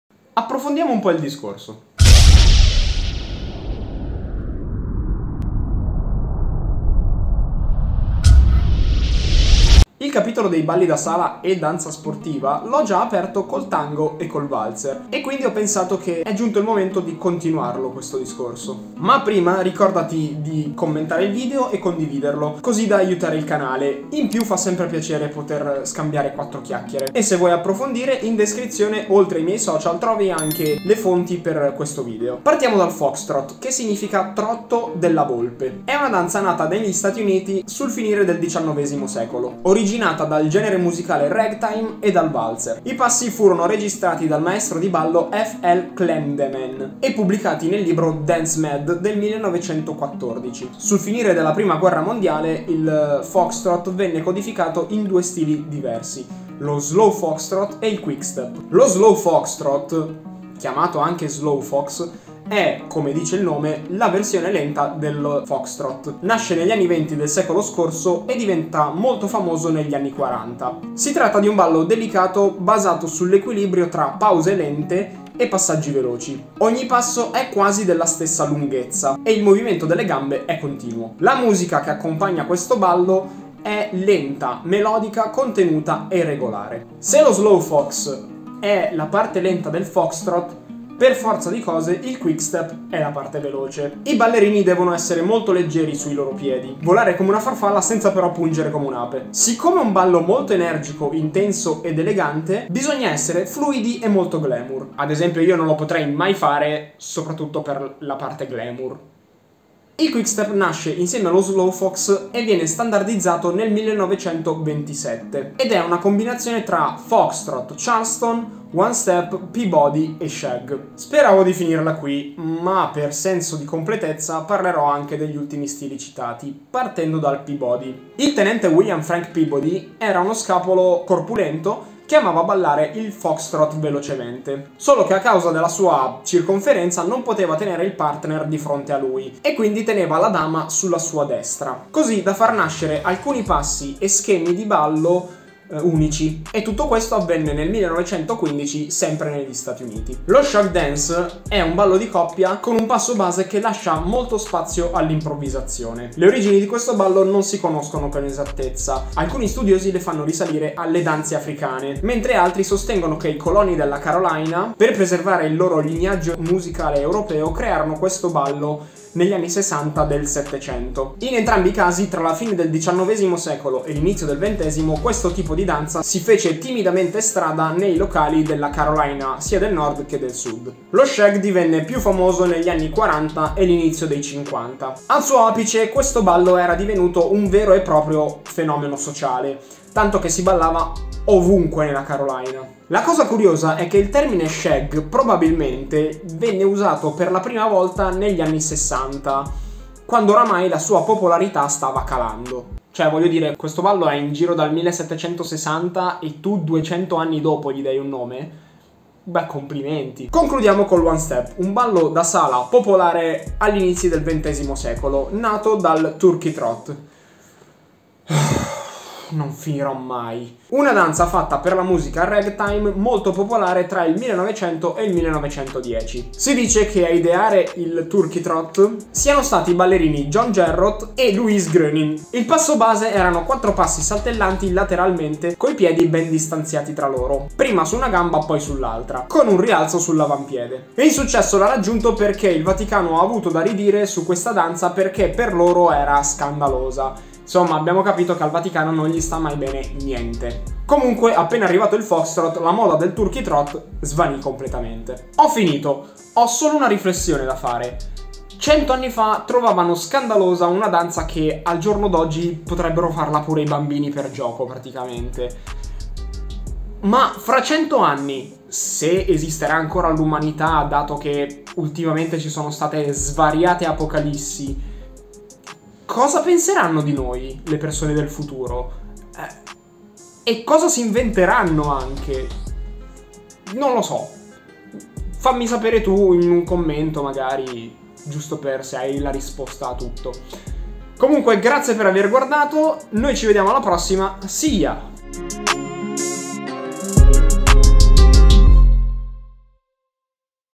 FOXTROT & i Balli da Sala
Dopo Tango e Valzer eccone altri di balli da sala

00:00 | Intro
00:32 | Foxtrot
00:58 | Slowfox
01:27 | Quickstep
02:01 | Peabody
02:30 | Shag Dance
03:30 | One Step & Turkey Trot
04:15 | Outro